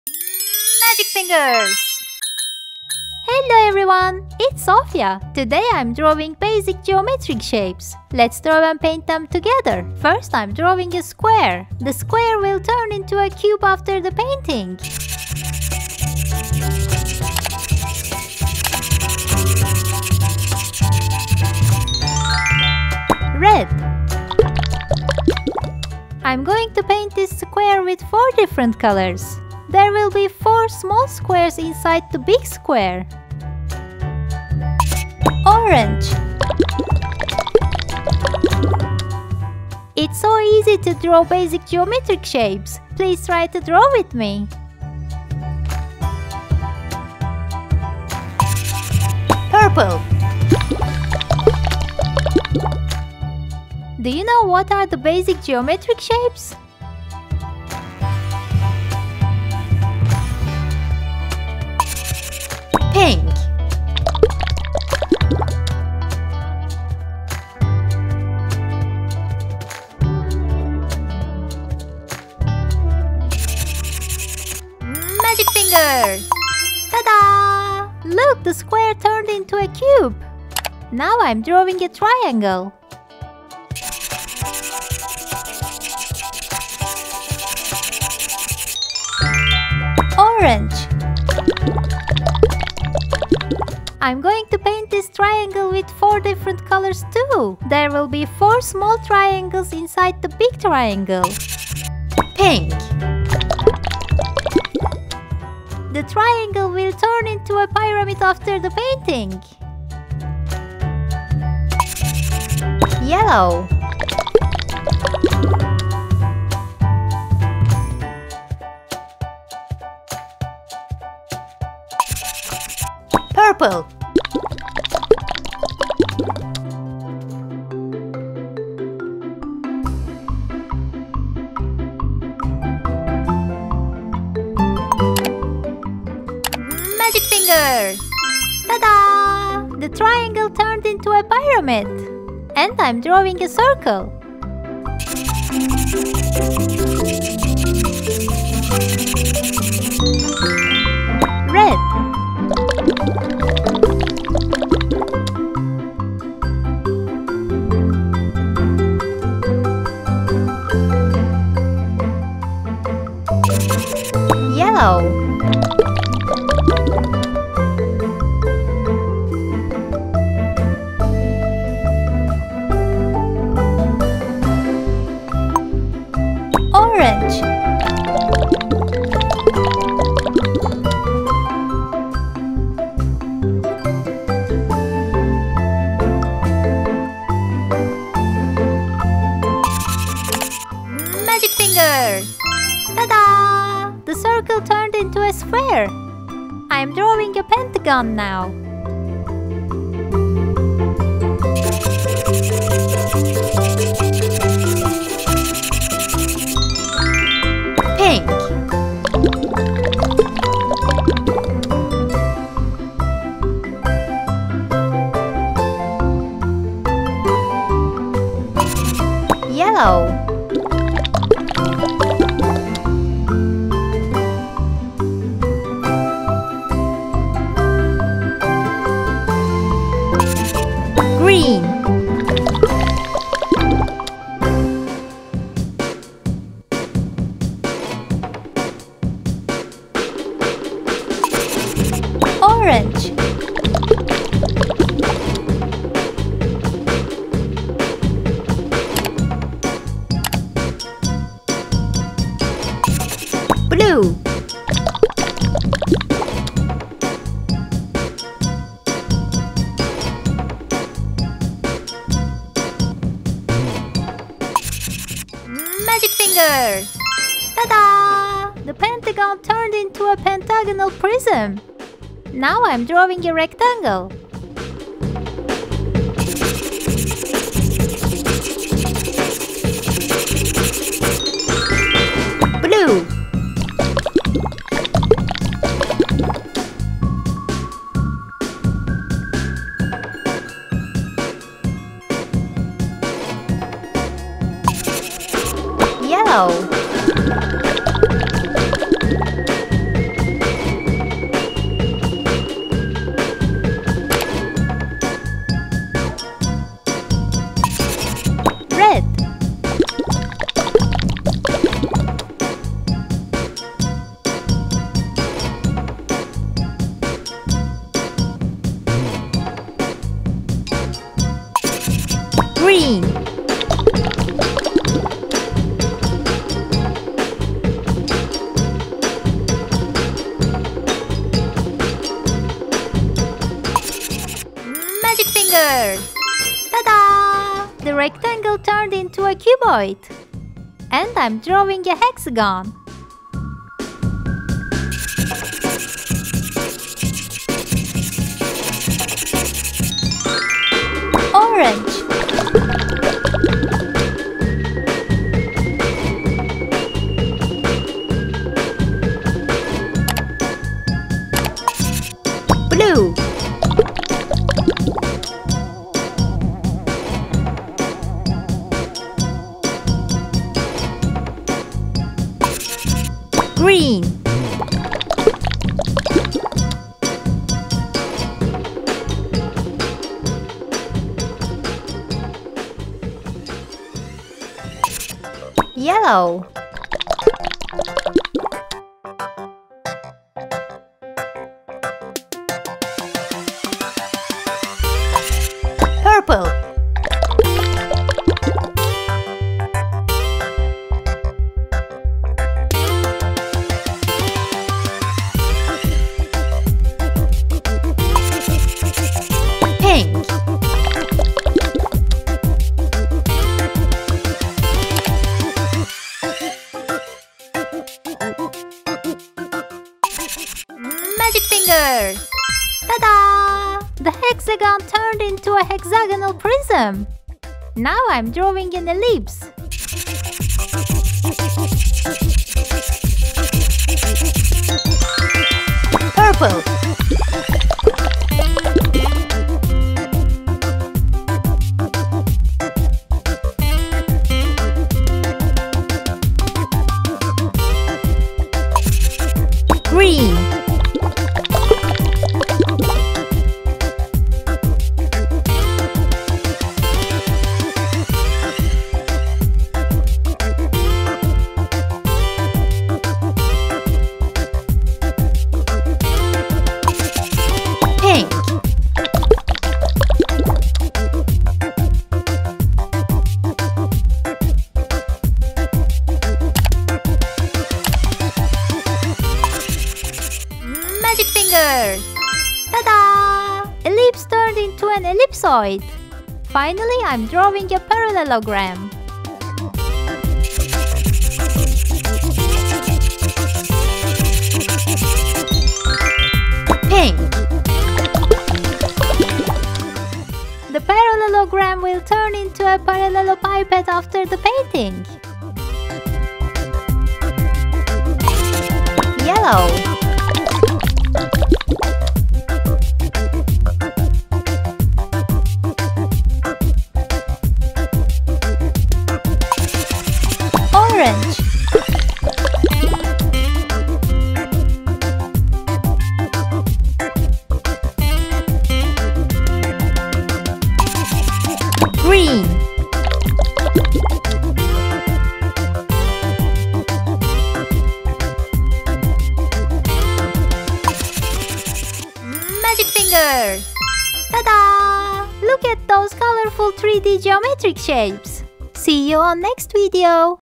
Bentuk Geometris Dasar Menggambar, Melukis, Mewarnai untuk Anak, Balita | Pelajari Bentuk 3D #333
Halo! Itu Sophia. Hari ini saya menggambar, melukis, mewarnai bentuk geometris dasar.

Anda dapat mempelajari bentuk geometris dasar dan bentuk geometris 3D dengan video ini.
Di video kali ini saya menggambar persegi, segitiga, lingkaran, segi lima, persegi panjang, segi enam, elips dan jajar genjang. Bentuk-bentuk ini berubah menjadi bentuk geometris 3D. Persegi menjadi kubus, segitiga menjadi limas, lingkaran menjadi bola, segi lima menjadi prisma segi lima, persegi panjang menjadi berbentuk kubus, segi enam menjadi prisma heksagonal, elips menjadi elipsoid, jajar genjang menjadi paralelopiped.
Silakan mencoba menggambar bentuk geometris dasar ini bersama saya...

00:00 | Pendahuluan
00:25 | Persegi
01:18 | Segitiga
02:19 | Lingkaran
03:14 | Segi Lima
04:29 | Persegi Panjang
05:37 | Segi Enam
07:04 | Elips
08:04 | Genjang

Seni ♡

Belajar Dasar-Dasar Menggambar dan Mewarnai untuk Anak dan Balita ♡
Cara Pelajari Warna ♡
Video Pembelajaran Terbaik ♡